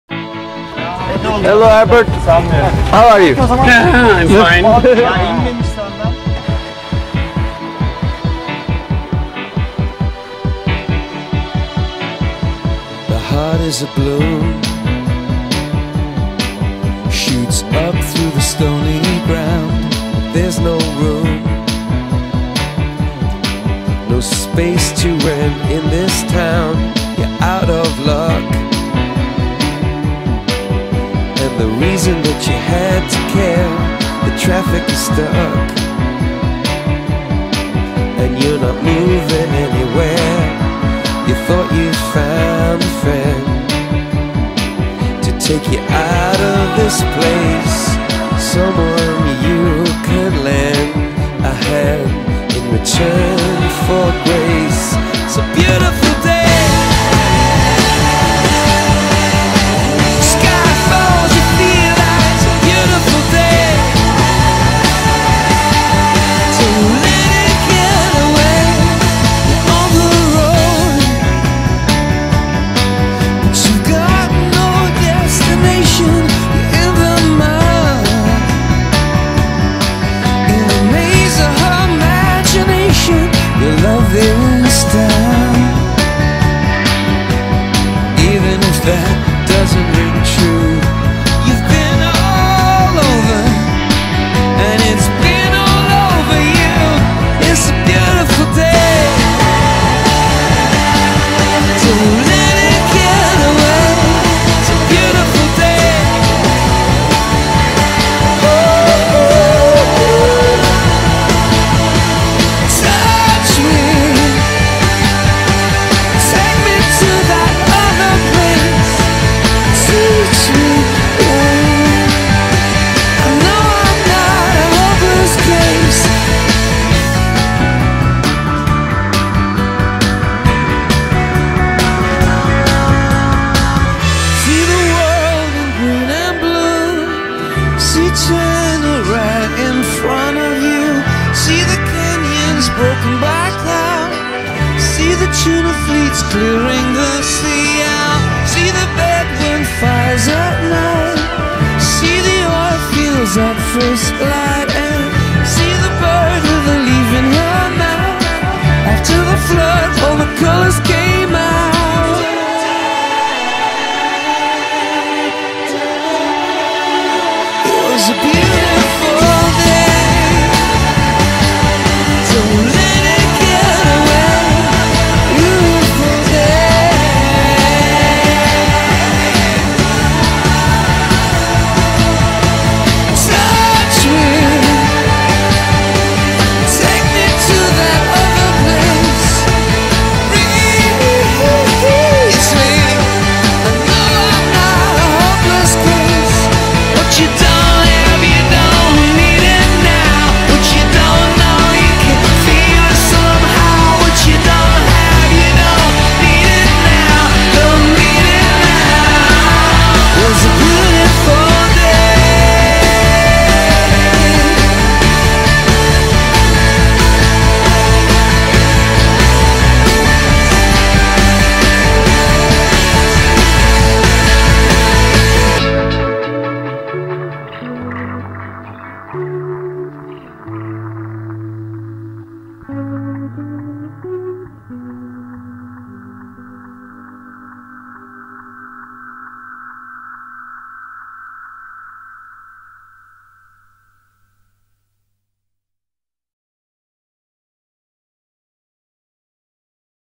A Beautiful Day
A nice spring afternoon flight along the ridge in Kas, Antalya. Life can be so beautiful!
Recordings of Olcay Unal and Ugur Yavas, Tandem pilots of Naturablue.
If you watch carefully you can see Herbert in the swimming suit at "Bulut" Take Off.